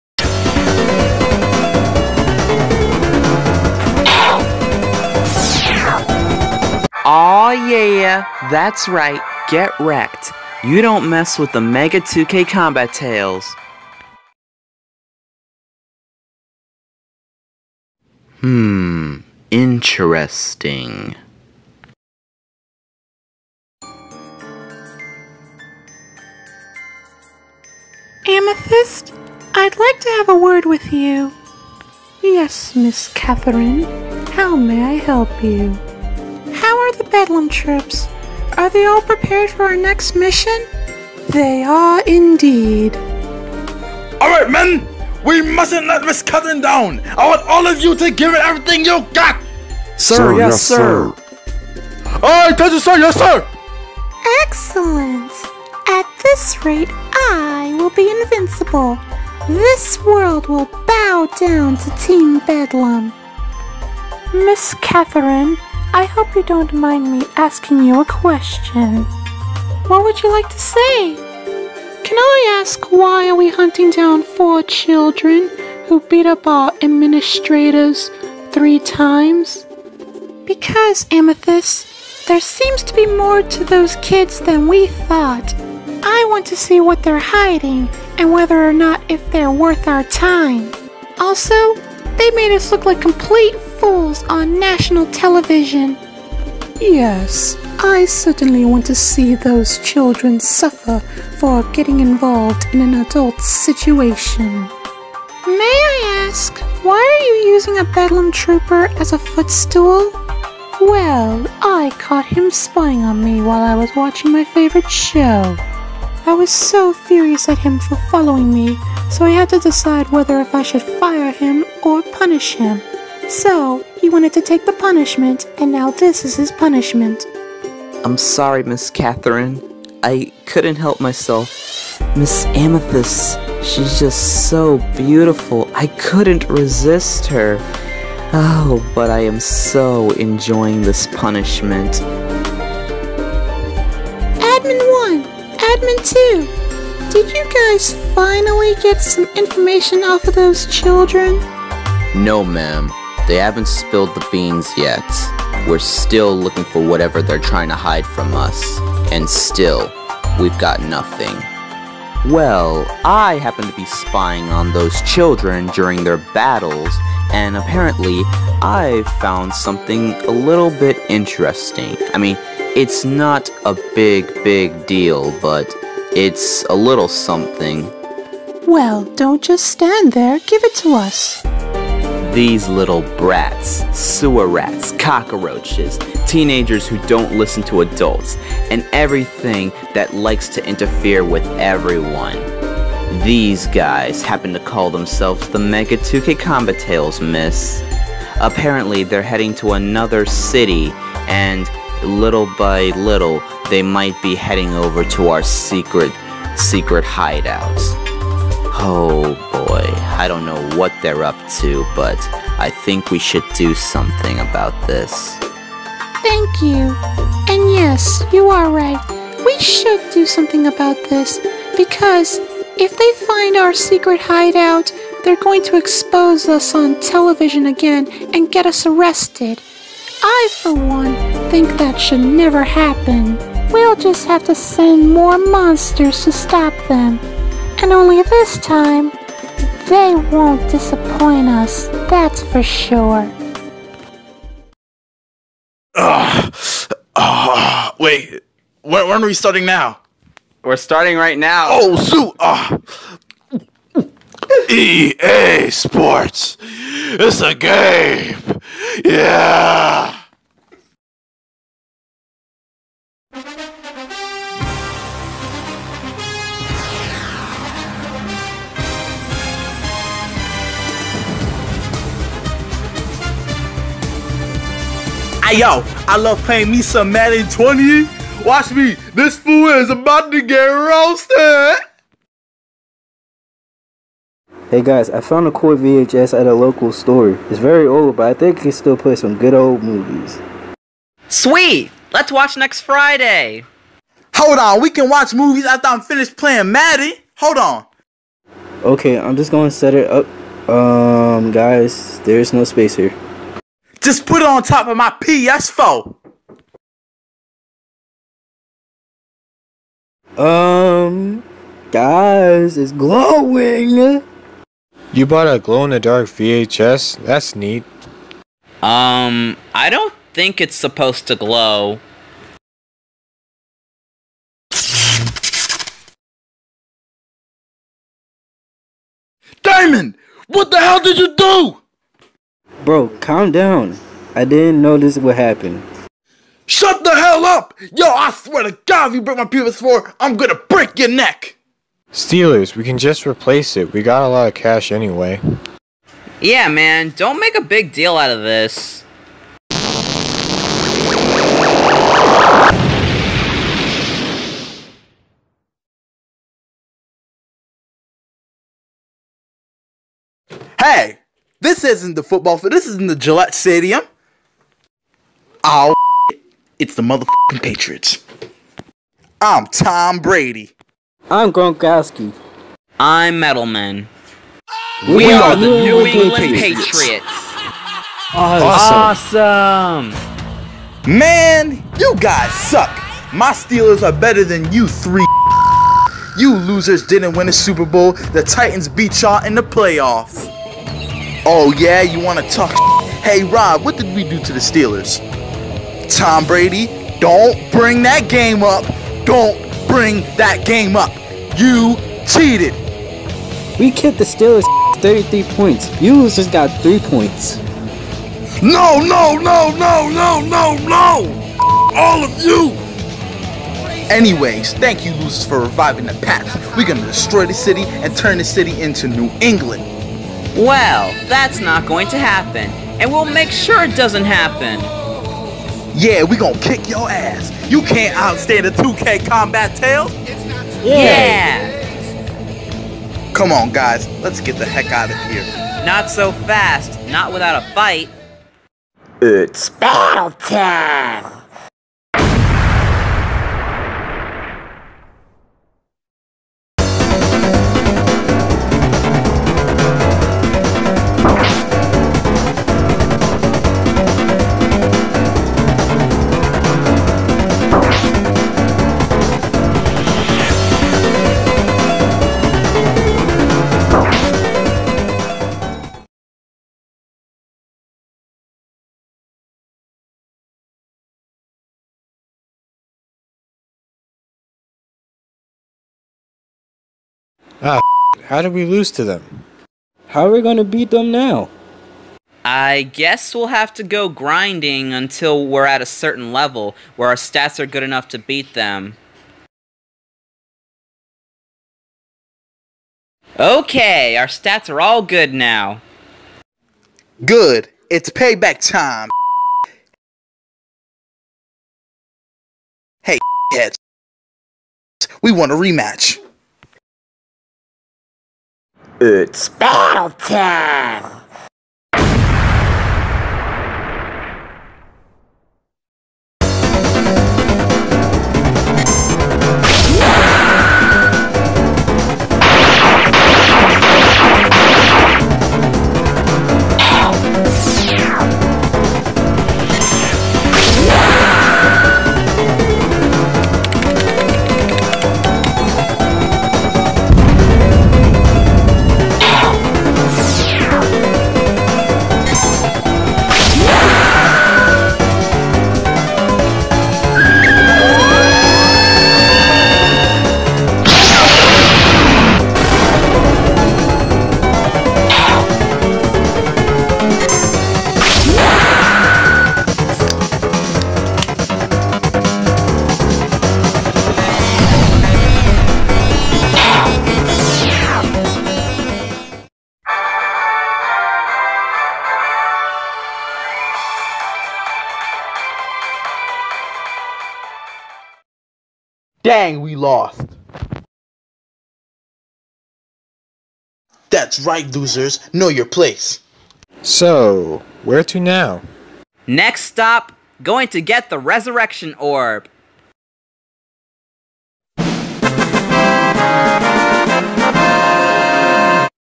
Mega2Kombatales Adventures part 5
The Mega2Kombatales still continue their adventure but to their surprise they come across the New England Patriots.